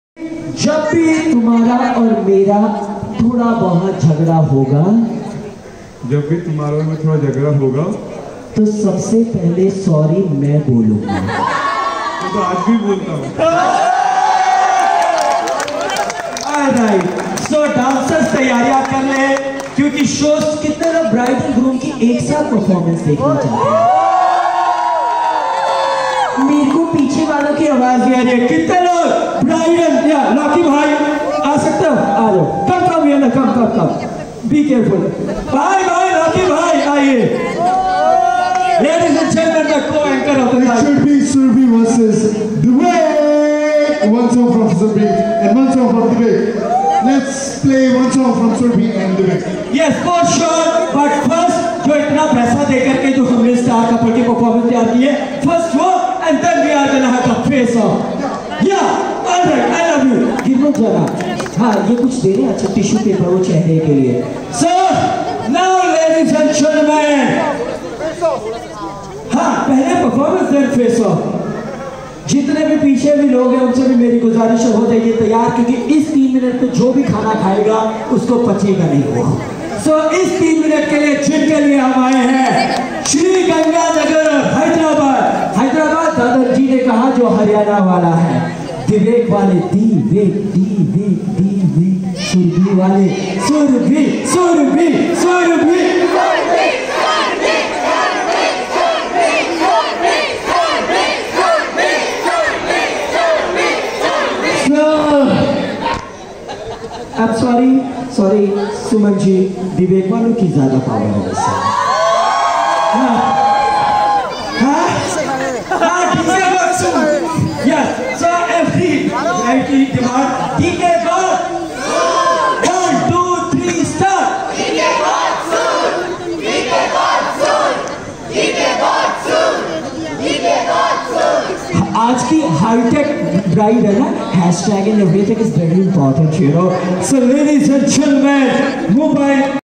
Wedding Anchor Girish Sharma Announcing The Bride Groom Performance | Destination Wedding Anchor
Wedding Anchor Girish Sharma Announcing The Bride Groom Performance | Best Destination Wedding Anchor